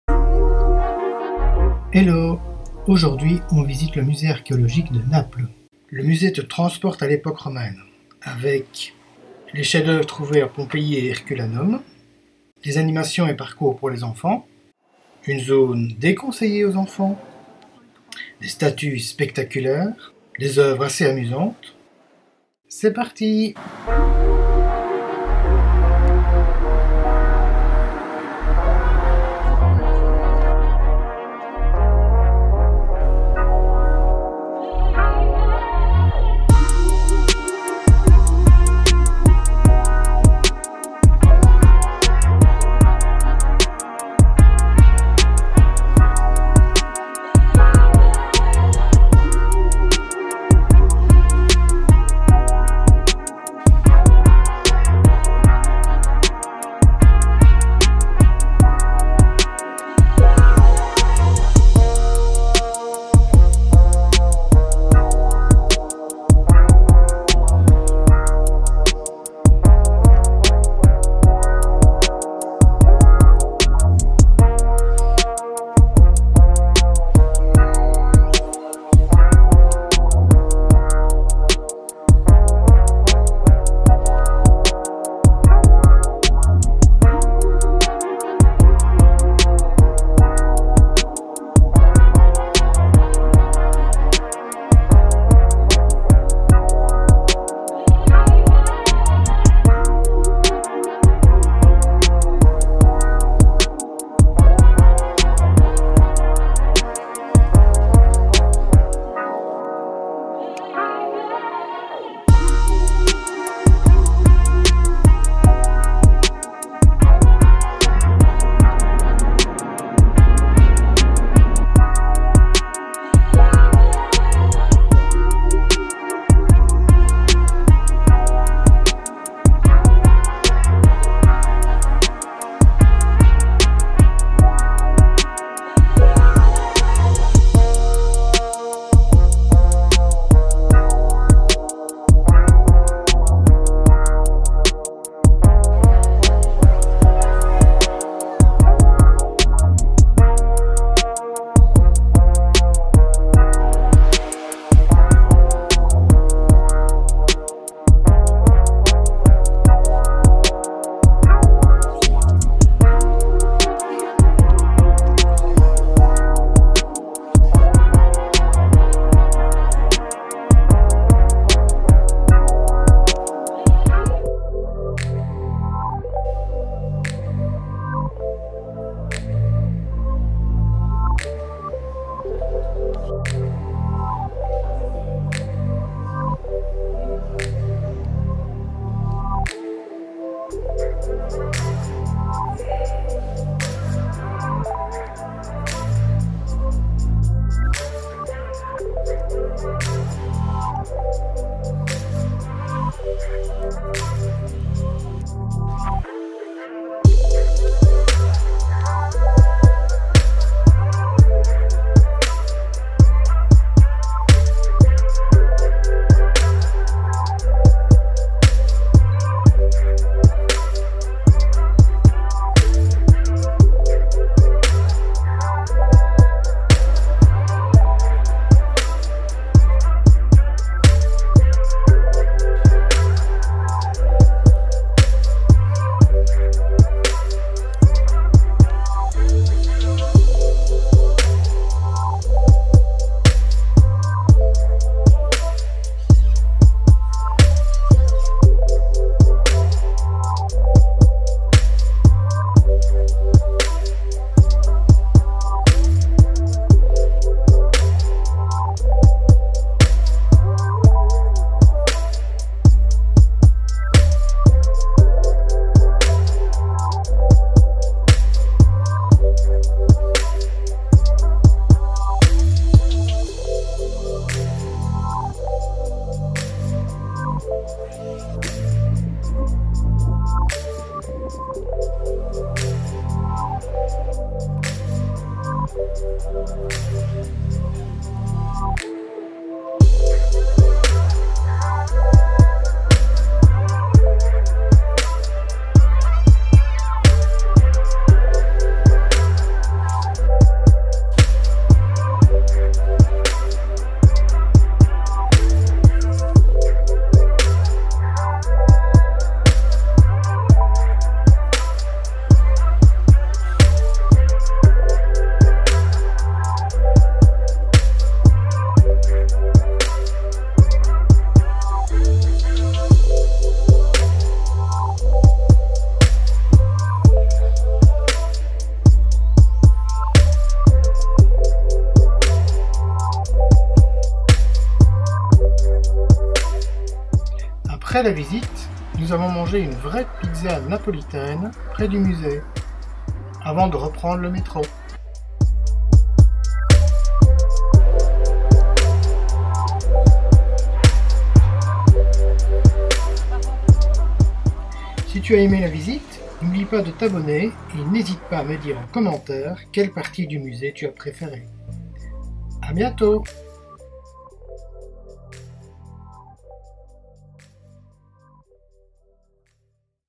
Visiter Naples : le Musée Archéologique National (MANN)
Je t'emmène visiter le Musée Archéologique National de Naples (Museo Archeologico Nazionale di Napoli - MANN), un des plus beaux musées archéologiques du monde.
Il est surtout célèbre pour les trésors de Pompéi et d'Herculanum qui y sont exposés (peintures, mosaïques, sculptures...).
Tu y trouveras aussi les impressionnantes statues greco-romaines de la collection Farnèse et le Gabinetto Segreto (cabinet secret), qui cache l'art érotique découvert à Pompéi et Herculanum.
Il y a aussi des parcours et des animations pour les enfants : quand nous y sommes allés, c'était sur le thème "Star Wars et la Mythologie".

Visite en détail du Musée archéologique de Naples sur le blog